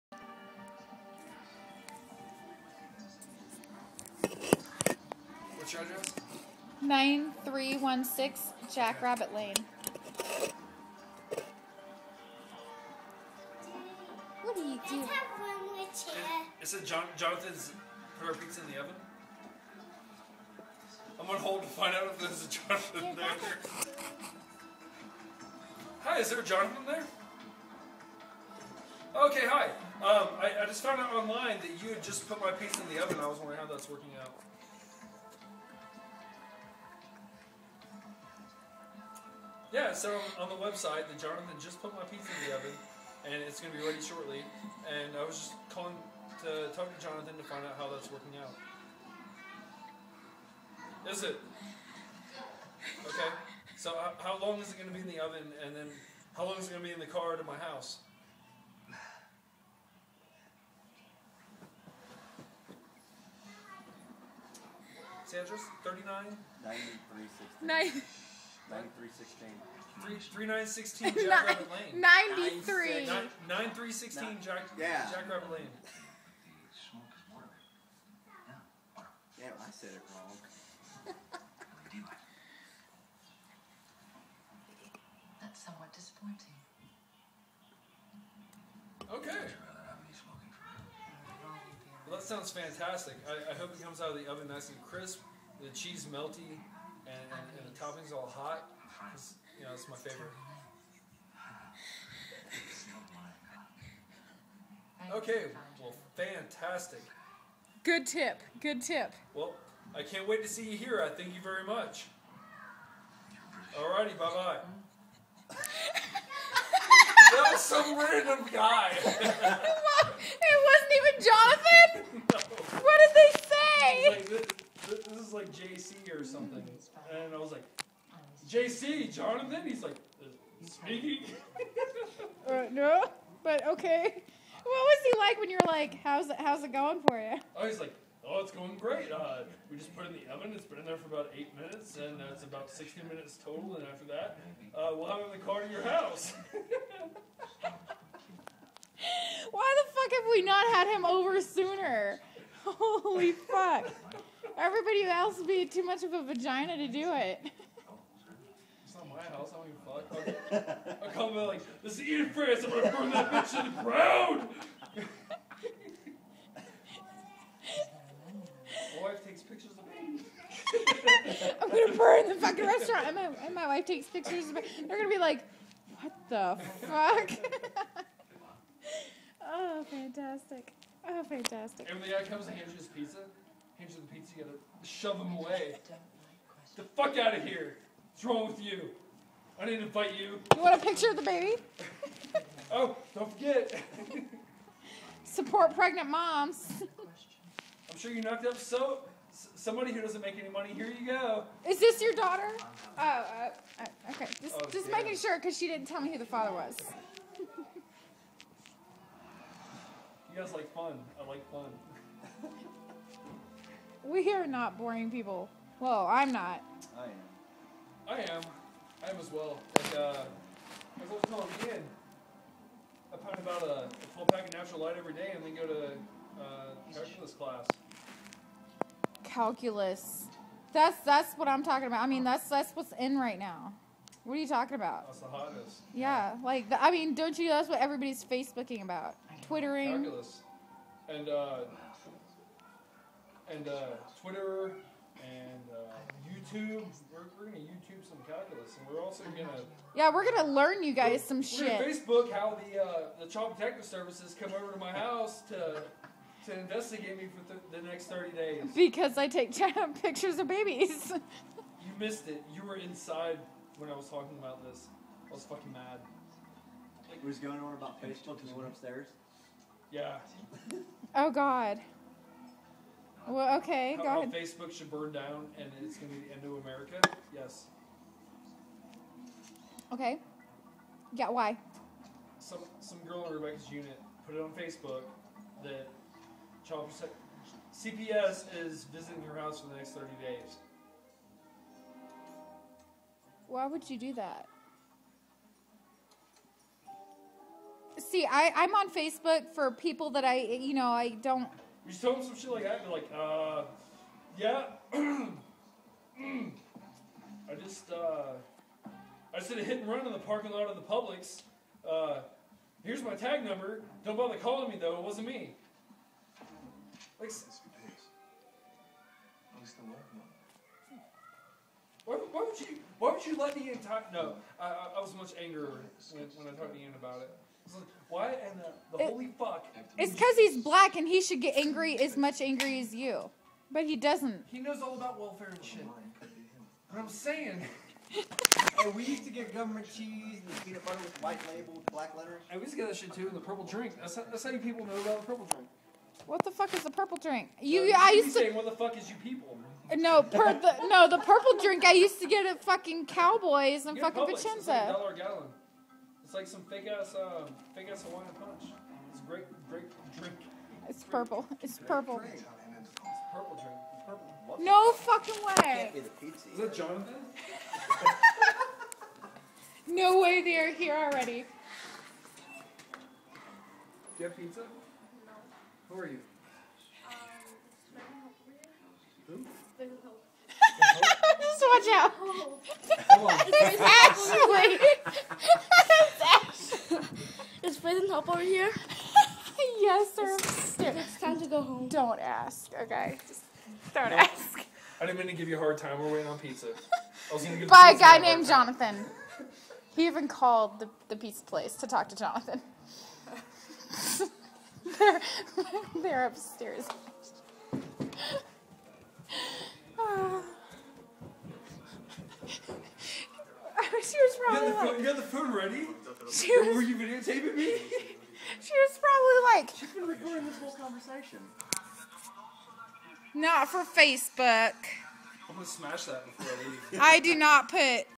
Pat calling Dominos
Calling Dominos to talk to Jonathan the guy who was making our pizza according to the website